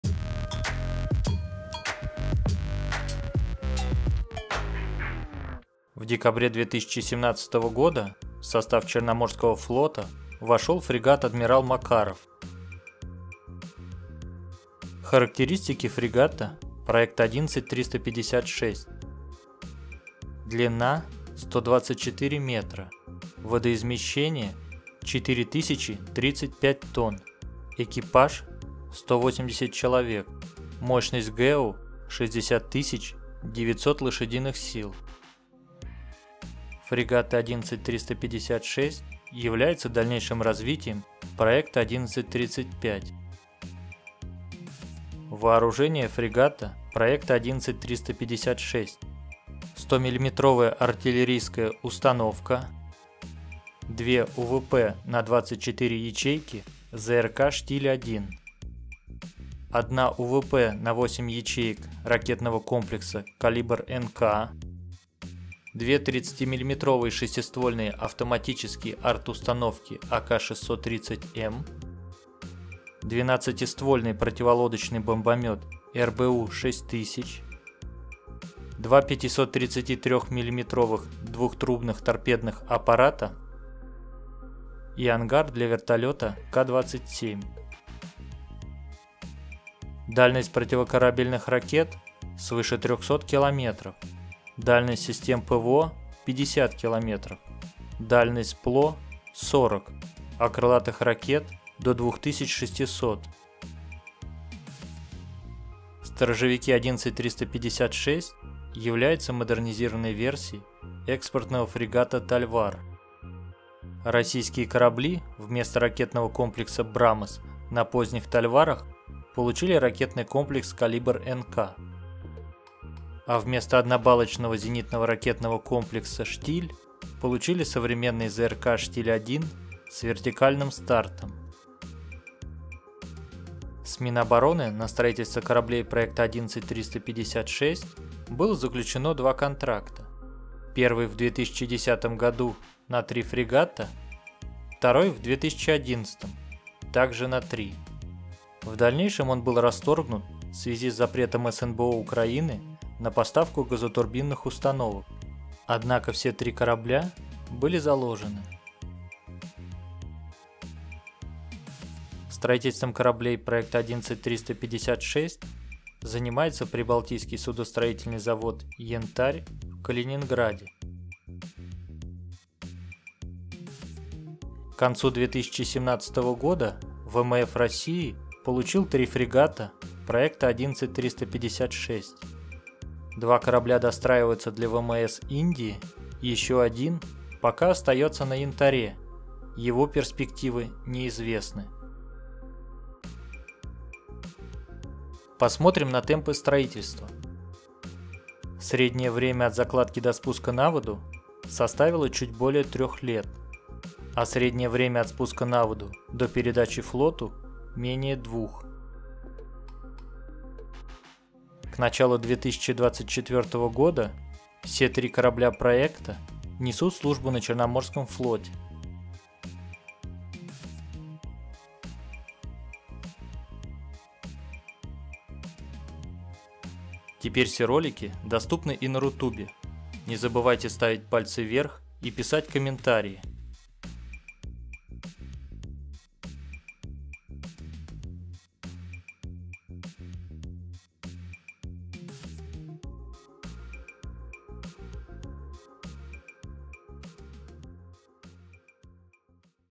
Обзор фрегатов 11356 "Буревестник". Обновление ВМФ России на 2024 год
Проект 11356 - многоцелевой фрегат 2-го ранга дальней морской зоны. Фрегат предназначен для ведения боевых действий против надводных кораблей и подводных лодок противника, а также отражения атак средств воздушного нападения, как самостоятельно, так и в составе соединения кораблей в качестве эскортного корабля. Всего российский флот получил 3 фрегата 11356 "Буревестник".

В этом видео:
00:00 начало
00:15 характеристики
00:45 вооружение
01:46 развитие проекта
02:14 контракты
02:41 судостроительные заводы
02:53 представители проекта
03:14 темпы строительства
03:30 распределение по флотам
03:47 Это интересно

Ссылки
Характеристики и вооружение: